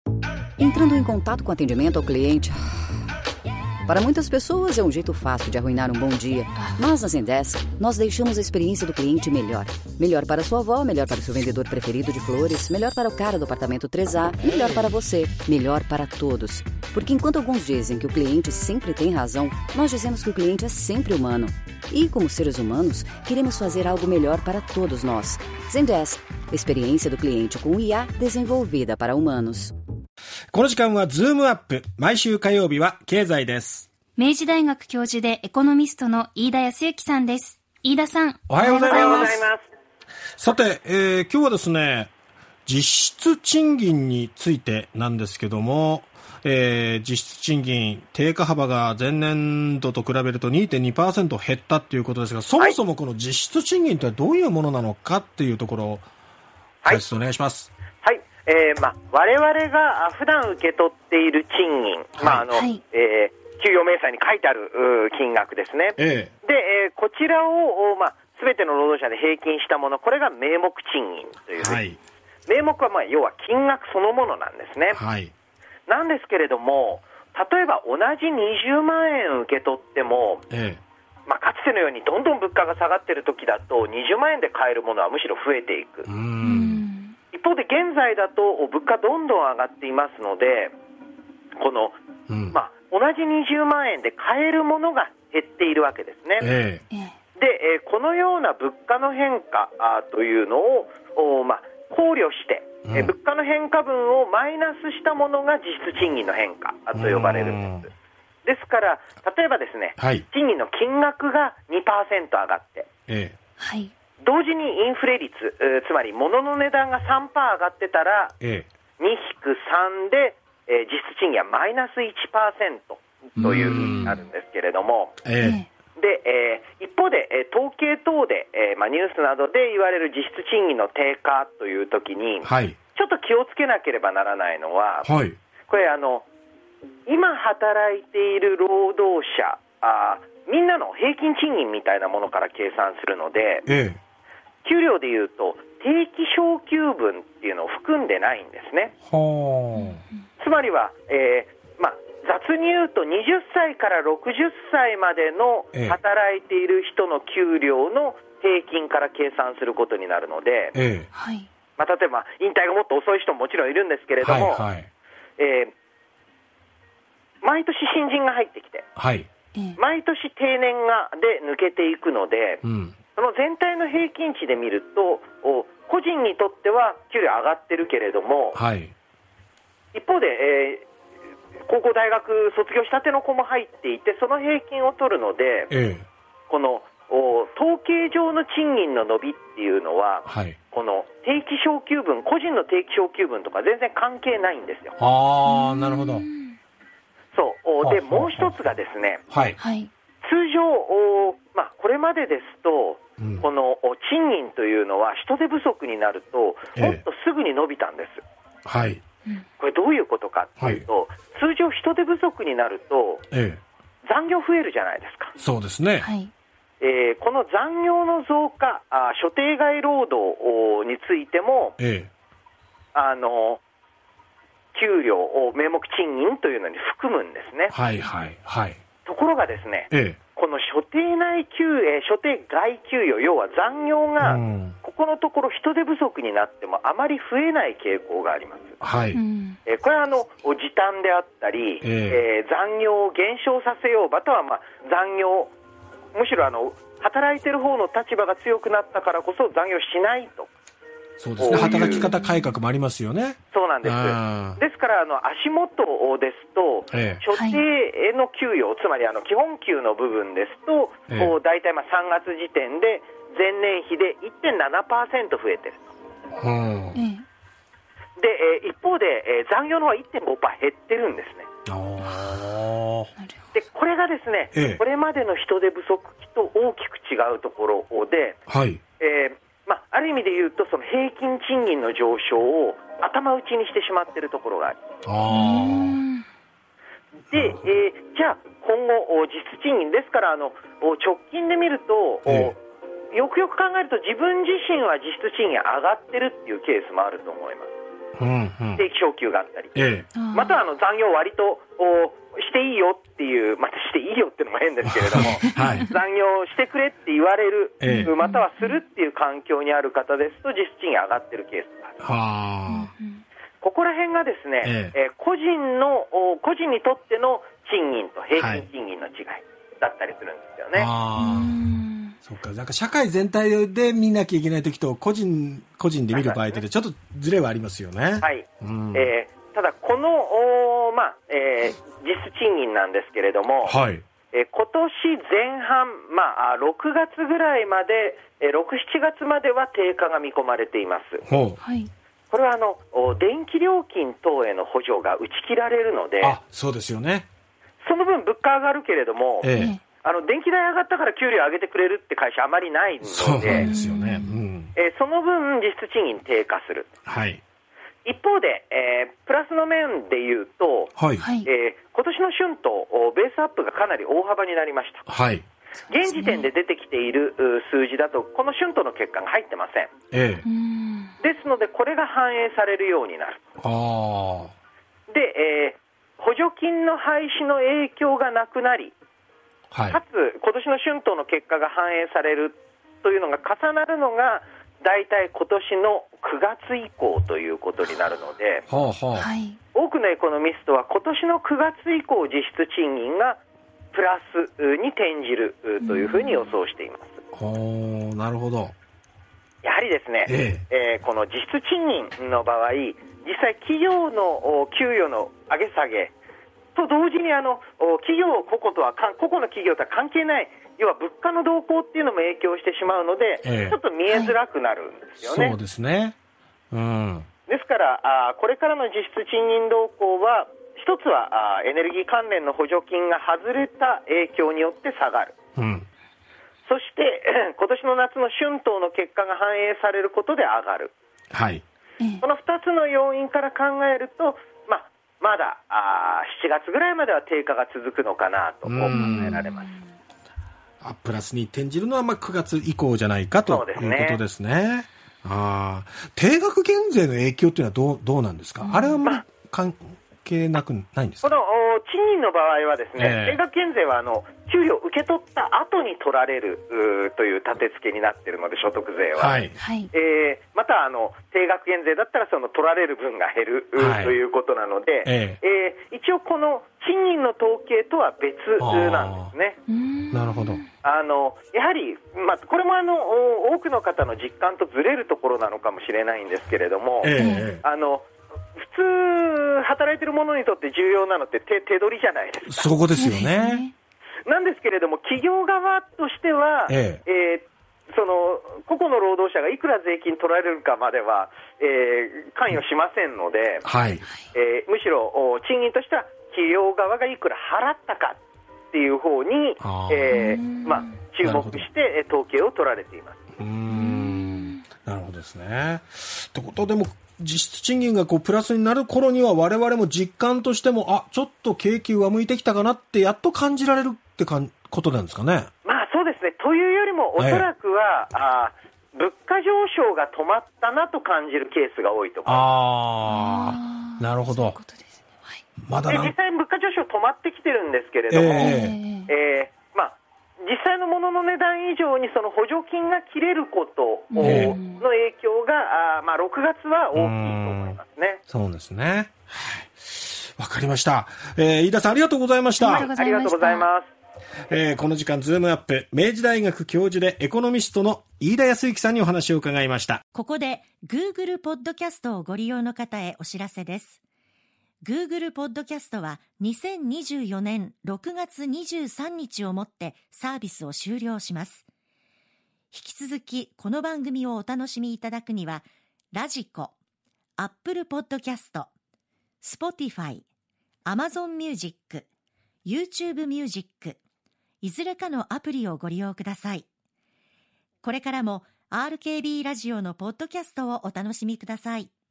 実質賃金、9年ぶりの低下幅
明治大学教授エコノミスト　飯田泰之